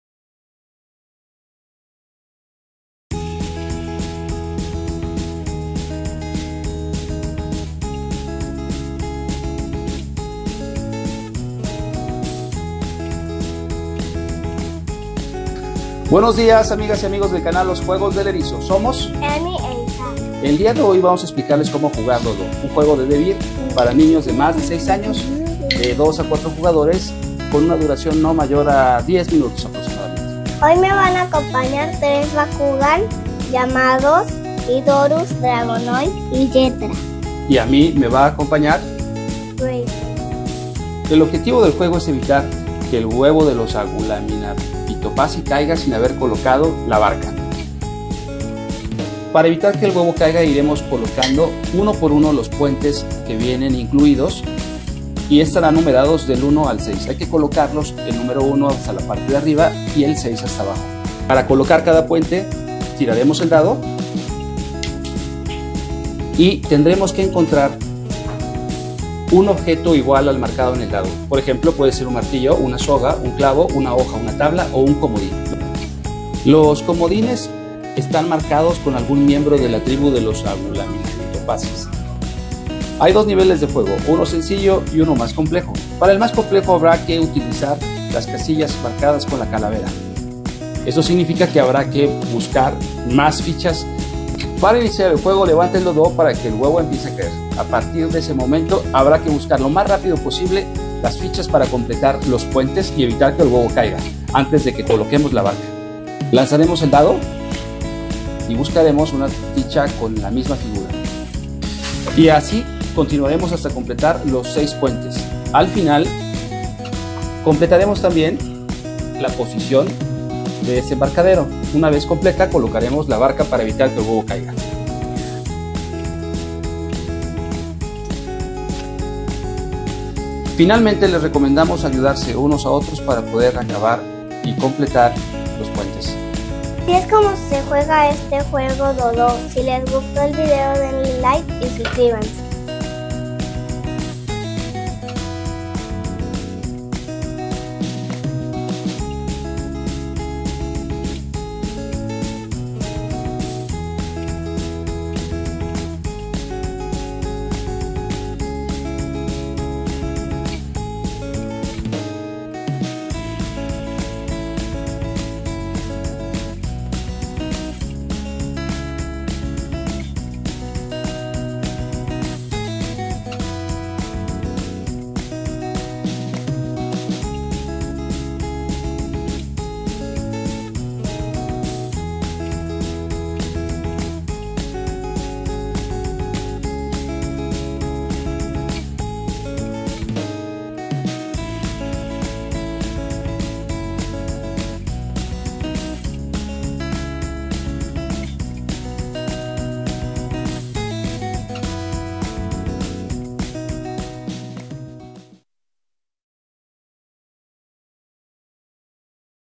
Dodo Devir | Cómo se juega.
Bienvenidos a los Juegos del Erizo, espacio para compartir reseñas de juegos, juguetes, libros y espacios lúdicos. En este nuevo video podrás aprender a jugar Dodo de Devir, un divertido juego coperativo en el que tendrás que salvar el huevo del Dodo.